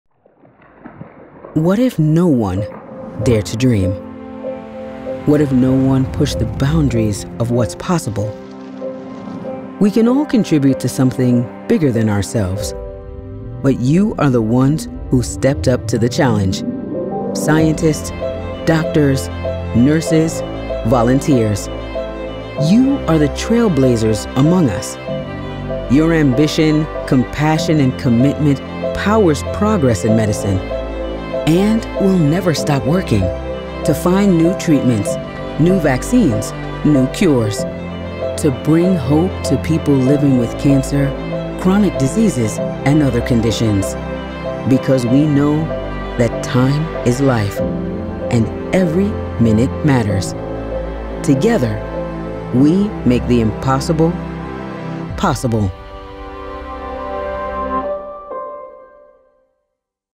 Clinical Trials Day 2024: The Trailblazers Among Us
This Clinical Trials Day, we recognize the progress in medicine thanks to clinical trials and the people who help make them possible, the trailblazers among us.